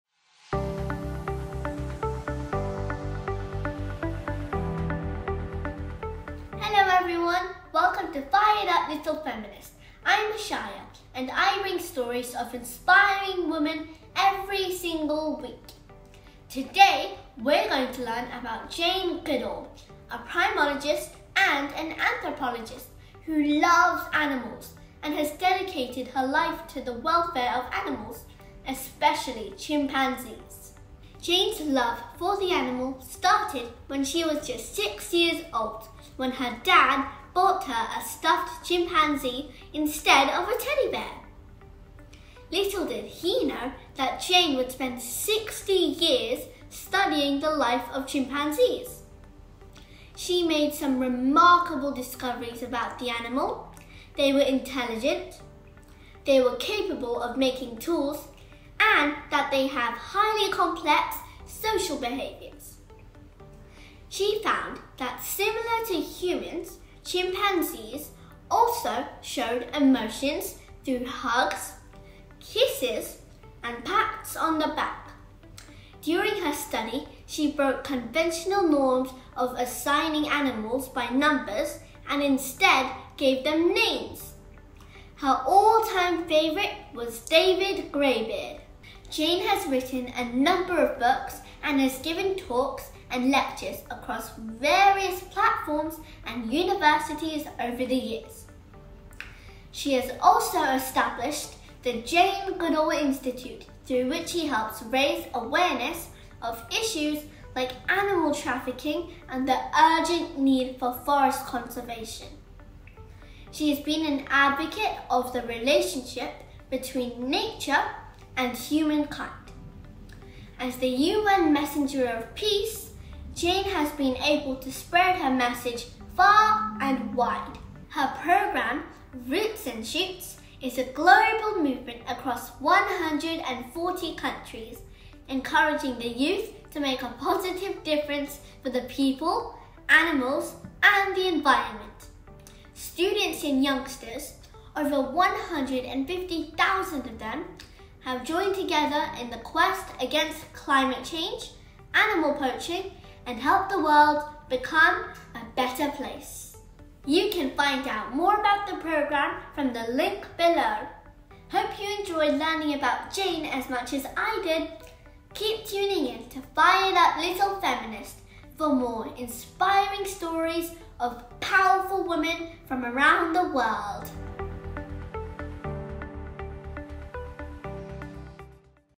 Jane Goodall l World Renowned Primologist and Conservationist
Today, we're going to learn about Jane Goodall, a primilogist and anthropologist, who loves animals and has dedicated her life to the welfare of animals, especially Chimpanzees!

Jane has written a number of books, and has given talks and lectures across various platforms and universities over the years.

She also established the Jane Goodall Institute through which she helps raise awareness of issues like animal trafficking and the urgent need for forest conservation.

She has been an advocate of the relationship between nature and humankind. As the UN Messenger of peace, Jane has been able to spread her message far and wide.